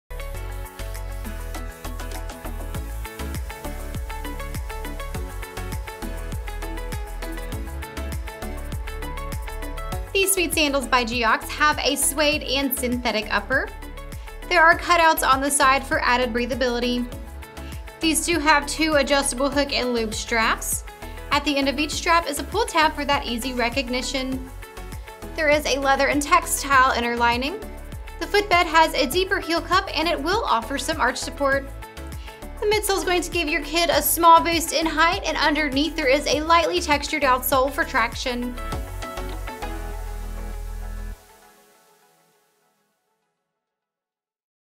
Geox Kids Strada 31 (Toddler/Little Kid) SKU: 9543779
Please click here to view a Geox&#174; Kids footwear size guide.
Your little one with walk, run, and jump in comfort in the Geox&reg; Kids Strada 31 sandal.
Leather covers the vamp and straps decorate the sides, with a toe bumper so their little toes stay safe on the playground.
Adjustable hook-and-loop straps over and around the ankle for a perfect fit.
Leather and textile lining and footbed.
Synthetic outsole.
Imported.
Geox&#8482; patented breathable shoe for active kids features a unique microporous membrane that absorbs and expels sweat while keeping water out.

Product measurements were taken using size 28 (US 10.5 Little Kid), width M. Please note that measurements may vary by size.